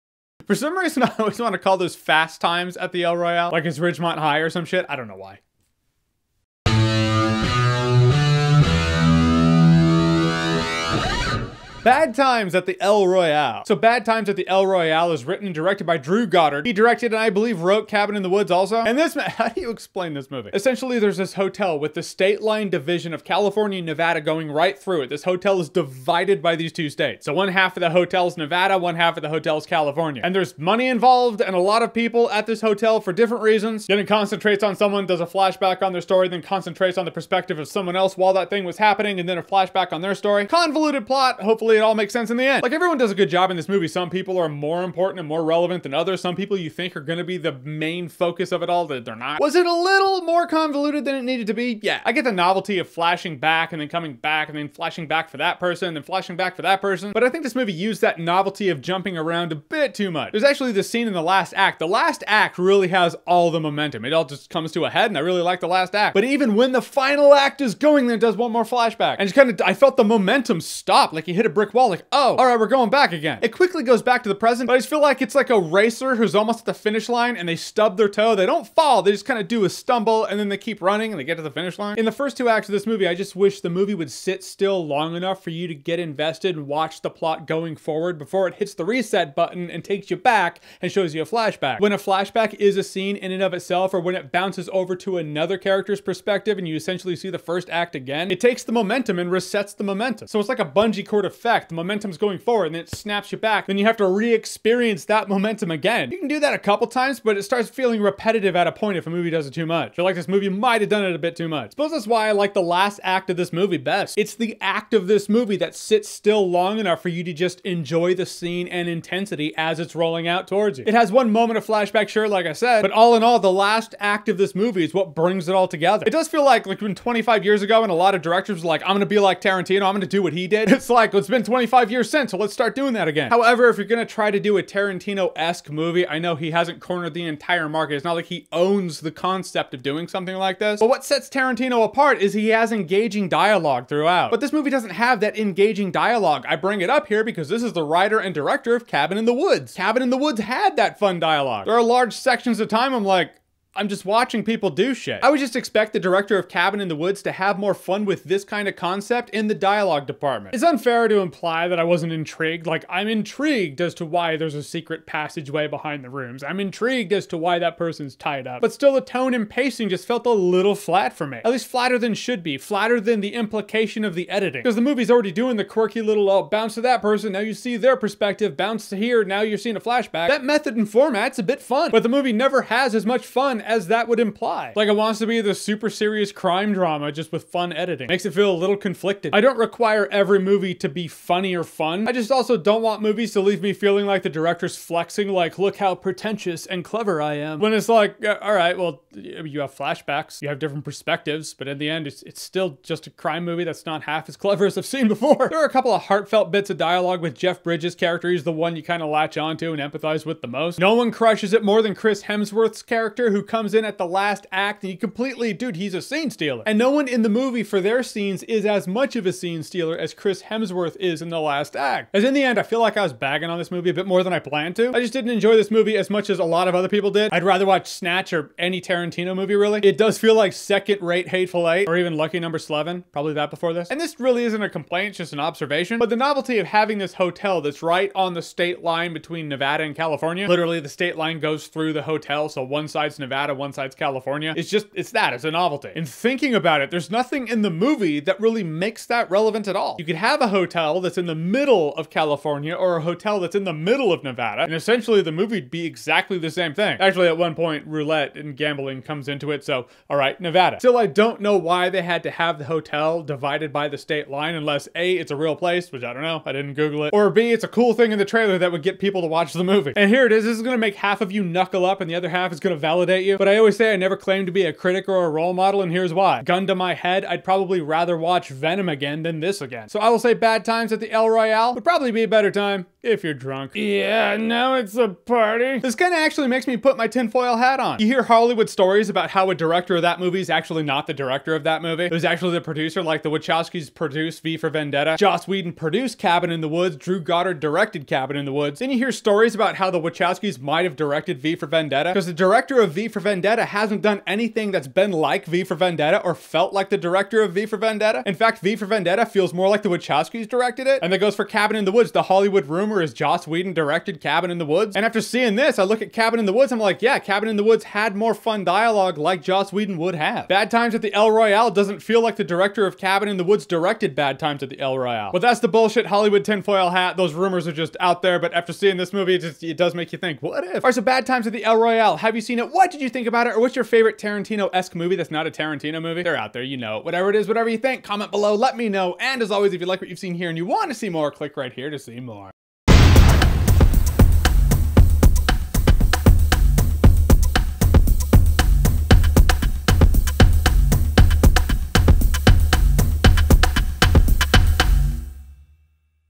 Bad Times at the El Royale - Movie Review
A mixed up crime drama involving a quirky hotel, a few strangers, and a lot of money. Here's my review of BAD TIMES AT THE EL ROYALE!